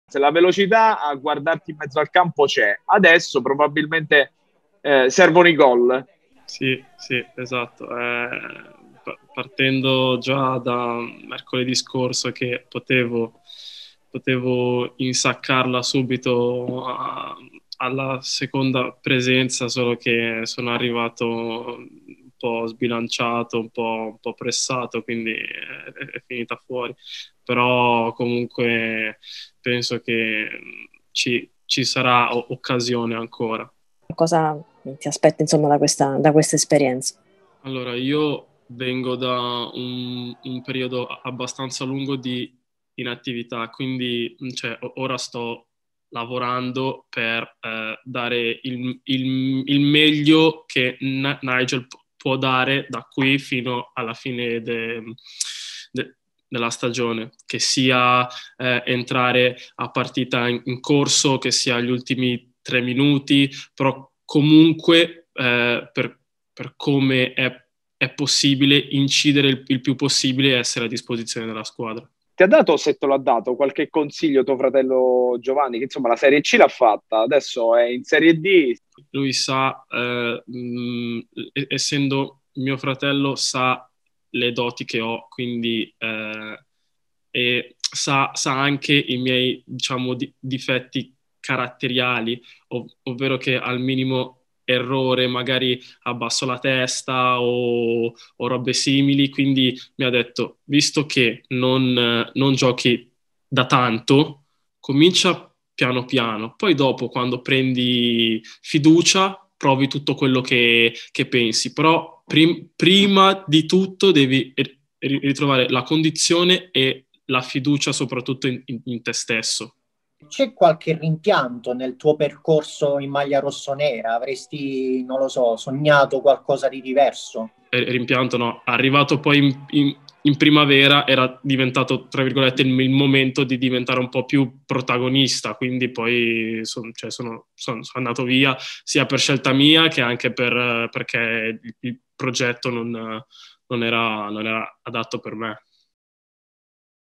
Teramo, Kyeremateng: “Lavoro per dare il meglio”
Altre grane in infermeria per il Teramo. Il tecnico Massimo Paci perde per problemi muscolari l’esterno Cappa e il centrocampista Viero che saranno indisponibili per il match con la Paganese assieme ai lungodegenti Lasik e Minelli e ai due calciatori che si sono infortunati contro il Potenza, Birligea e Di Matteo. Ci sarà invece uno degli ultimi arrivati, l’attaccante esterno Nigel Kyeremateng (italiano, di origini nigeriane) che si è presentato e ha parlato delle sue aspettative e dei suoi trascorsi nelle giovanili del Milan